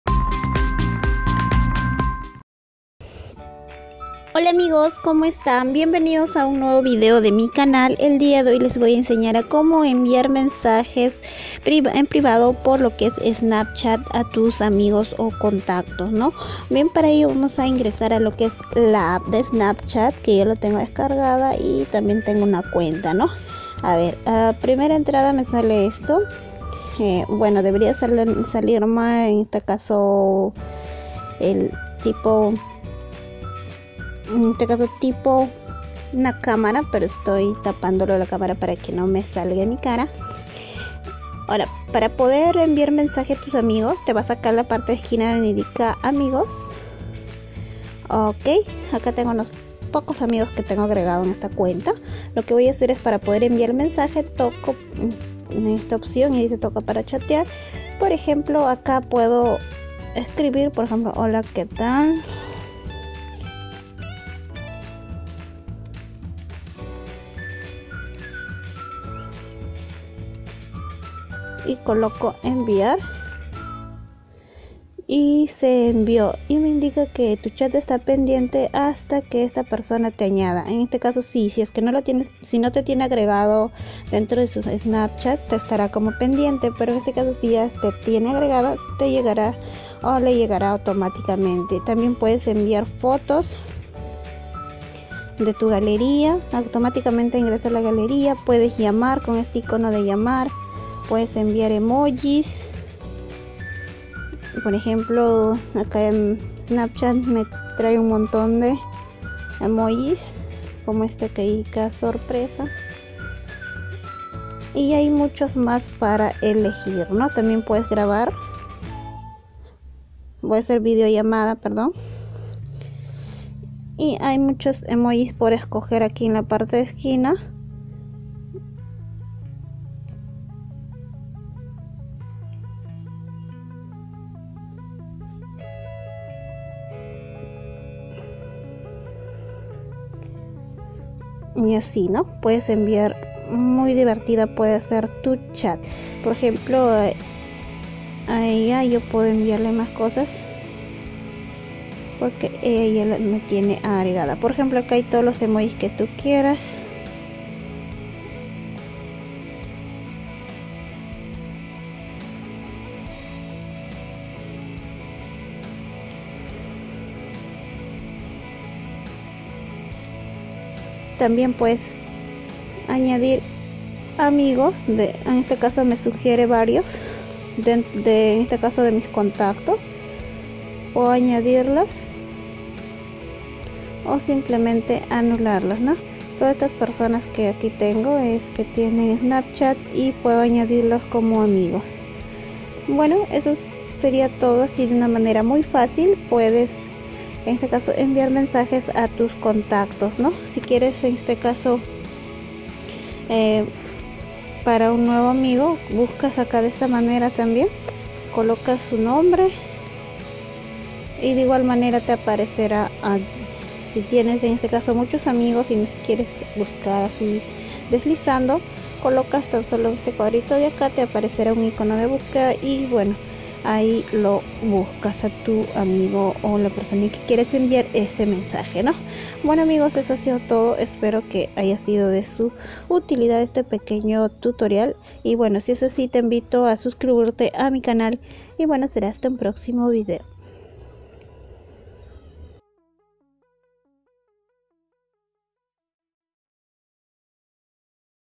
COMO ENVIAR MENSAJES PRIVADOS EN SNAPCHAT
Tutorial de como enviar mensajes privados a tus amigos o contactos mediante la app de snapchat de manera muy fácil y sencilla.